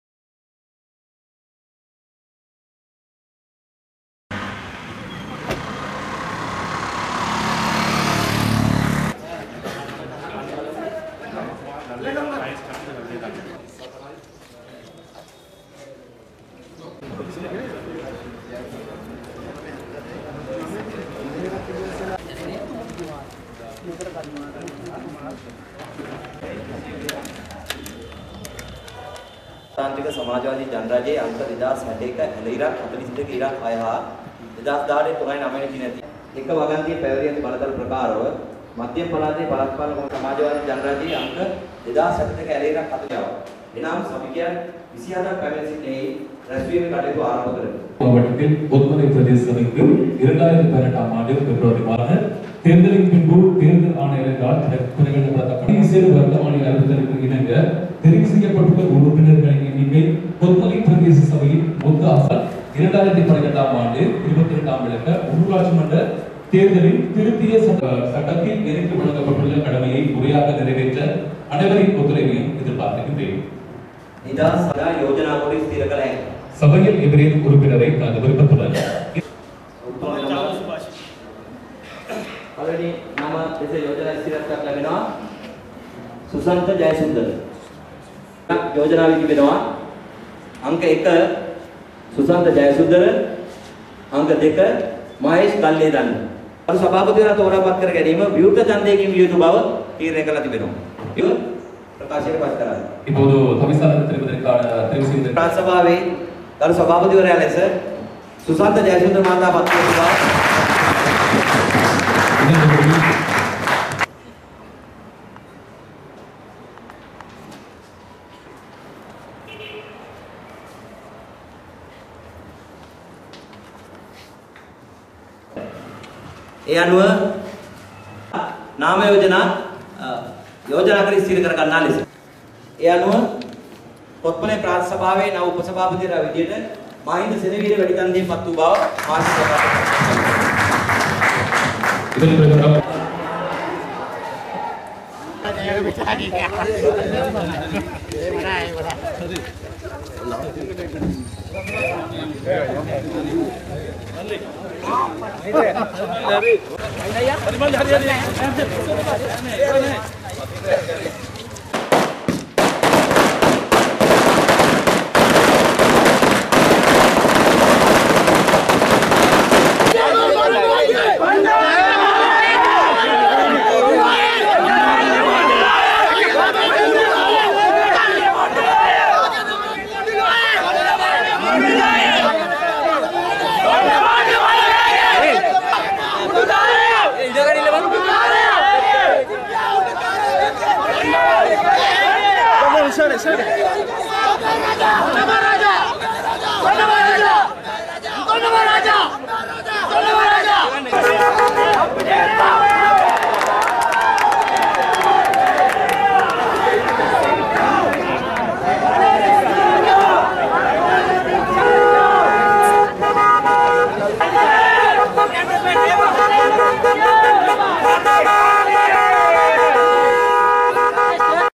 kothmale pradeshiya sabah chair person vice president elect
கொத்மலை பிரதேச சபைக்கான தலைவர், உப தலைவர் தெரிவு கொத்மலை பிரதேச சபை காரியாலயத்தில் இன்று இடம்பெற்றது.